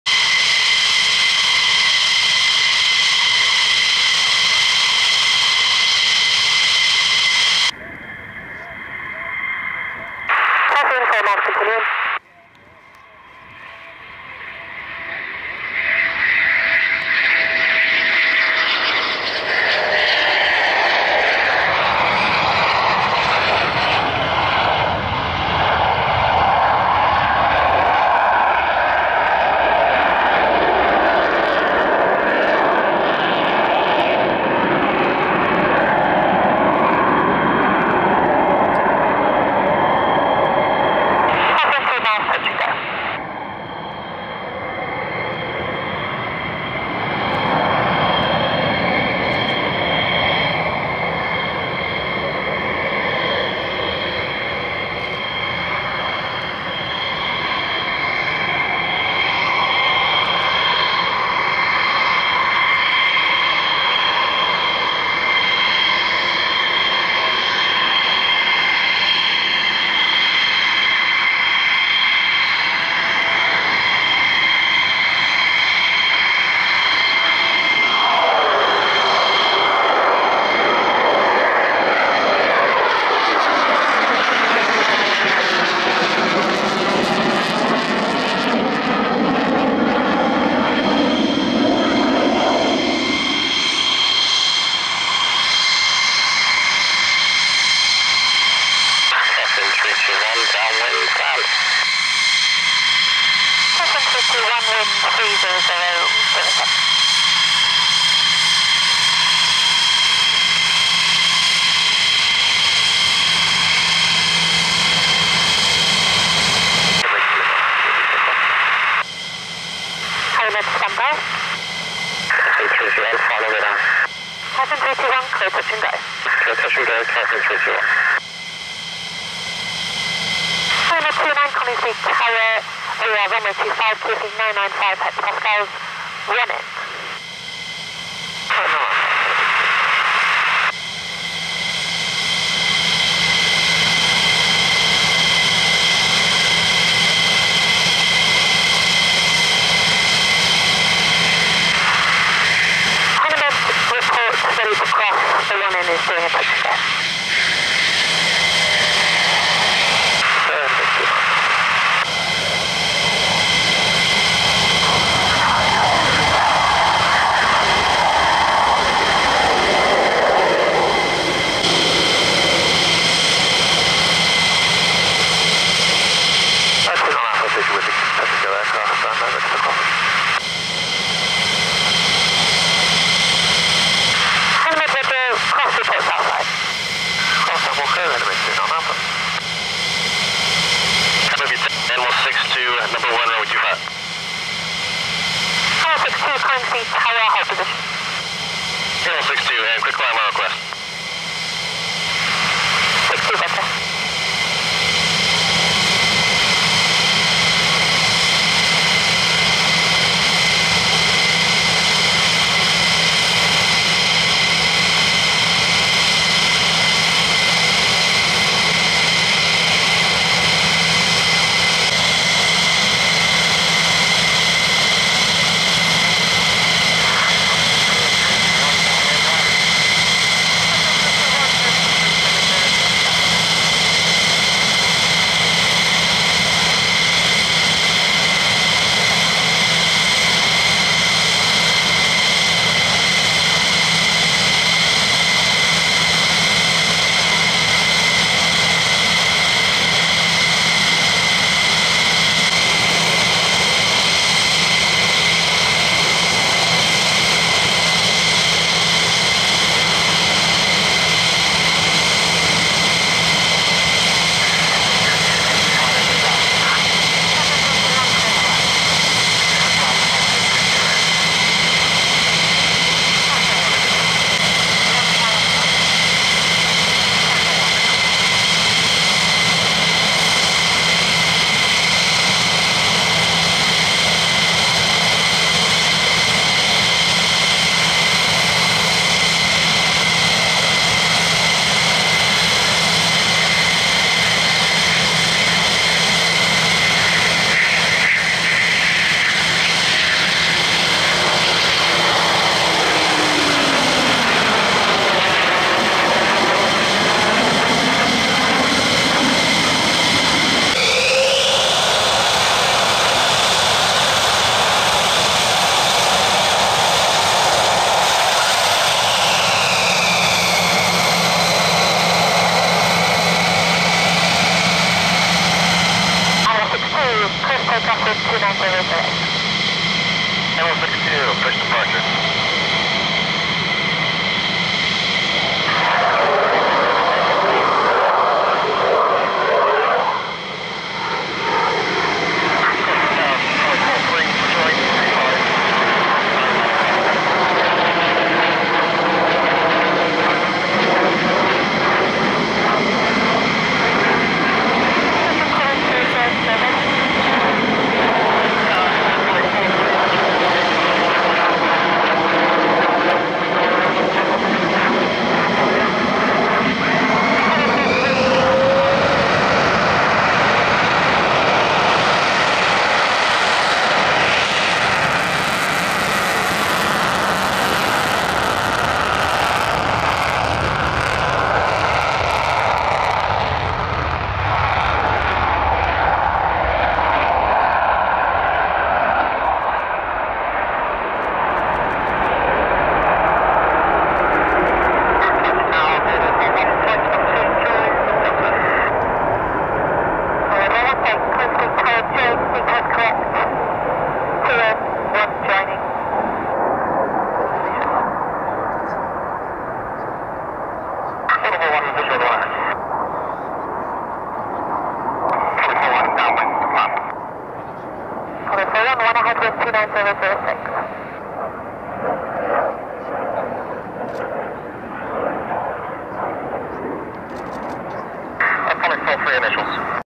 RAF CONINGSBY, USAF 48th FW F-15E LAUNCH 01-10-20
RAF CONINGSBY, USAF 48th FW F-15E OF RAF LAKENHEATH, LAUNCH RWY 25. SHOT IN 4K